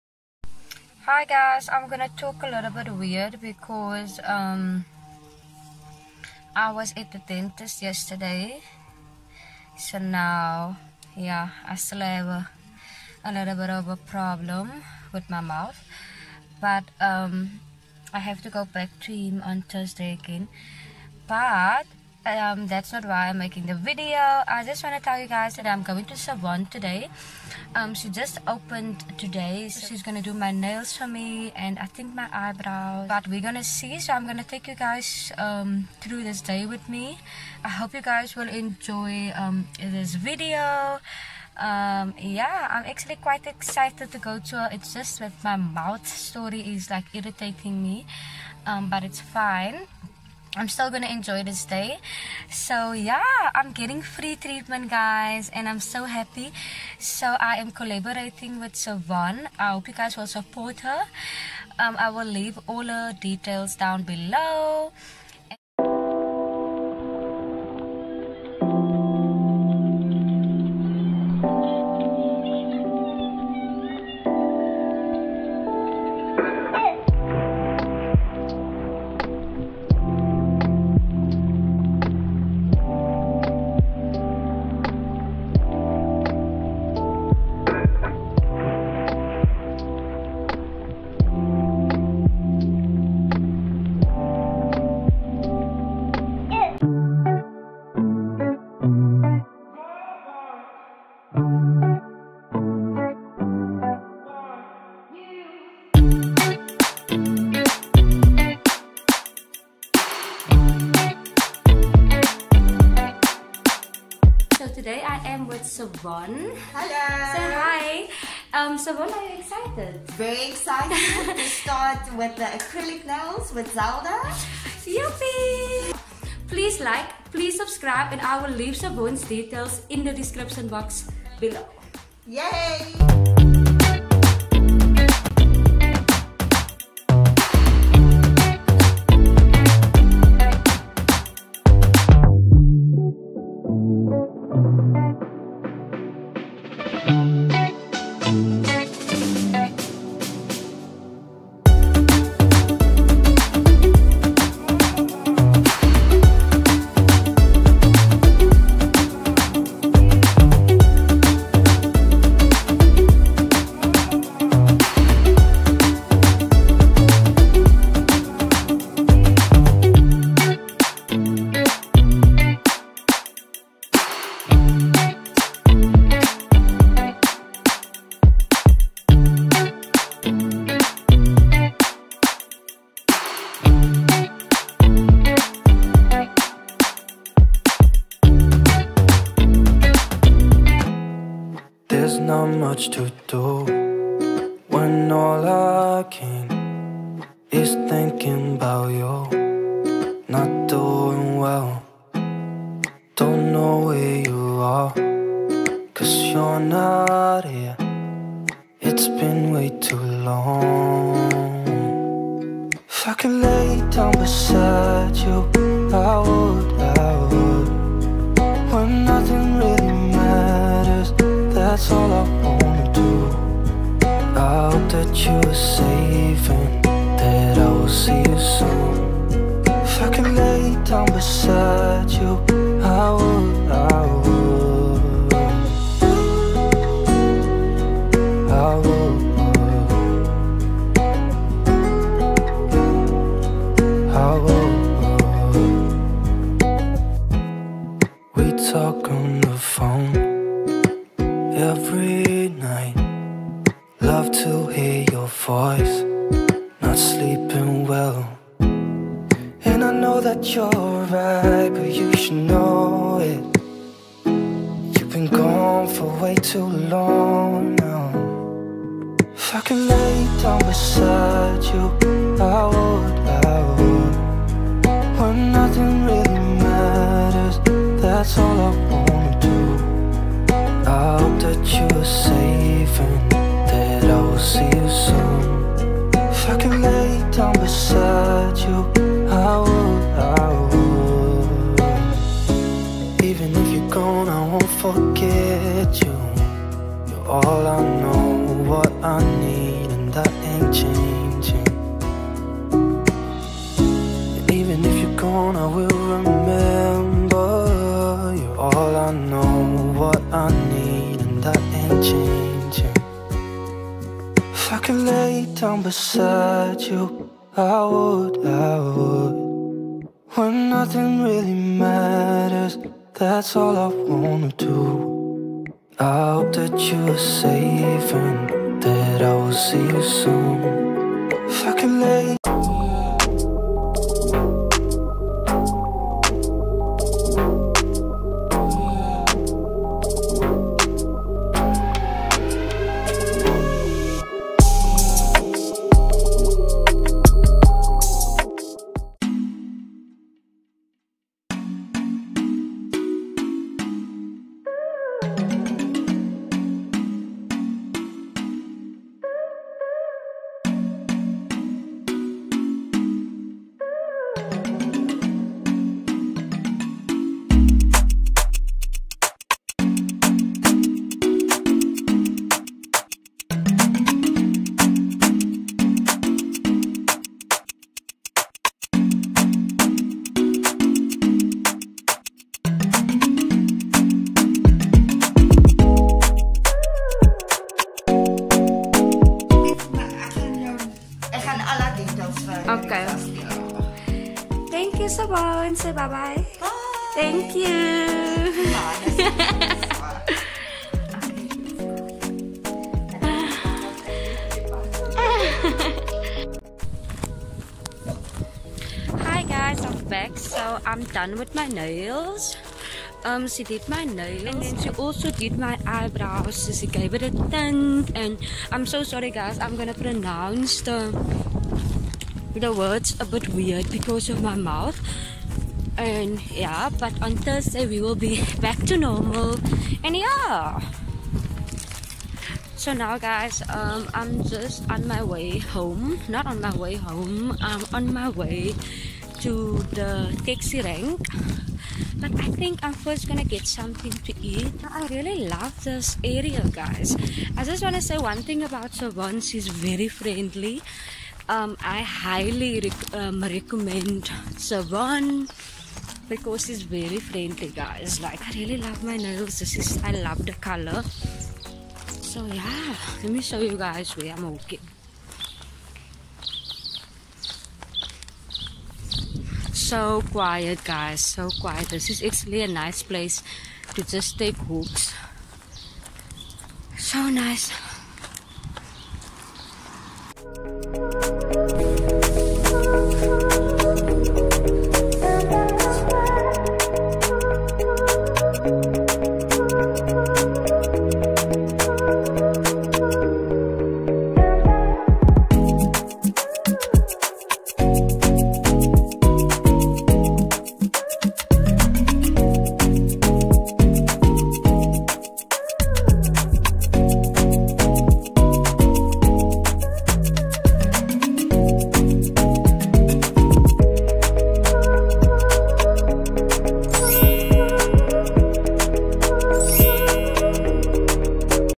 GO WITH ME TO MY NAIL APPOINTMENT ☺️|Acrylic Nails (Tips) 💅| Also got my brows threaded + Tinted
PLEASE WATCH THIS VIDEO ON THE HIGHEST QUALITY FOR BETTER VIEWING PLEASURE 🙏

Hey Darlings!

So I am back, this time with a Nail collaboration! So proud of Shavonne for opening her Nail salon, let's please support her by making use of her service and support her by spreading the word.

She didn't only do my Nails but she Threaded and tinted my eyebrows as well. I am so happy about the service I've recieved from her - Friendly and professional! And the place looks so beautiful and relaxing!